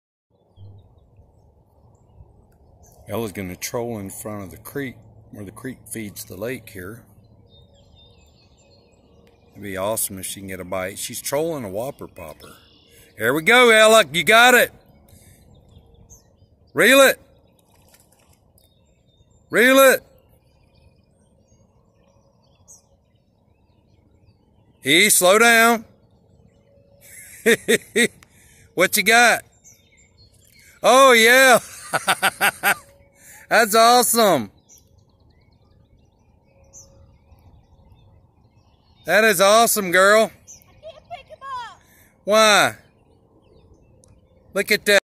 Ella Browning kayak trolling Whopper Plopper for bass, Shale Lake, Staunton, IL
Vacation during Covid Pandemic.  Great getaway at the Cottages at Shale Lake, where I've been staying while working in the area.  Beautiful lake with great fishing.